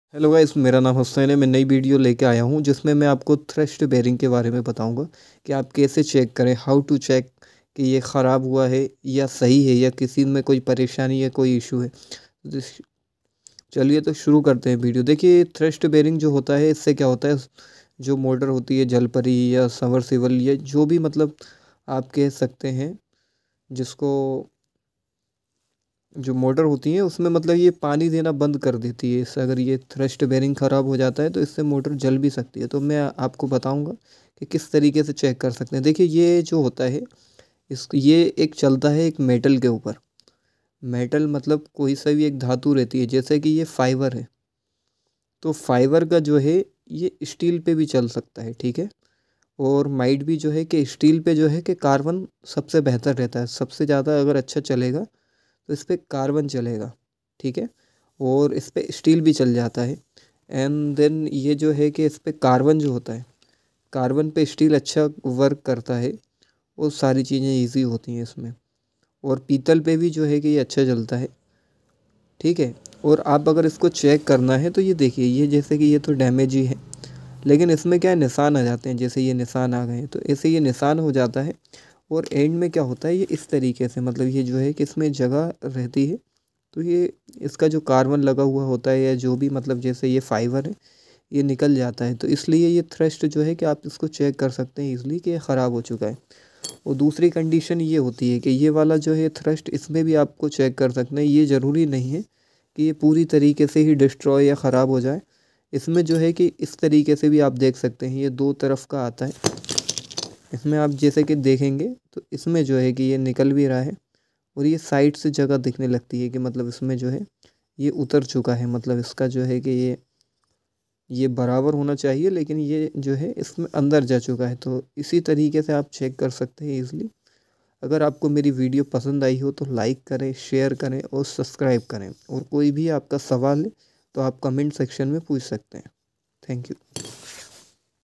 मोटर का थ्रस्ट बेयरिंग को कैसे चेक करे क्या दिक्कत है खराब हुए हैं या नही?Determine thrust bearings?
Determine whether or not to replace thrust bearings. ?
IN THIS VIDEO I'M GOING TO SHOW THE EXCAT TUTORIAL FOR THE ELETRICAL PROFESSIONALS OR BEGINNERS
IF YOU WILL FACE ANY TYPE OF ISSUE OR DOUBT IN THIS VIDEO#technical PLEASE ASK IN COMMENT SECTION.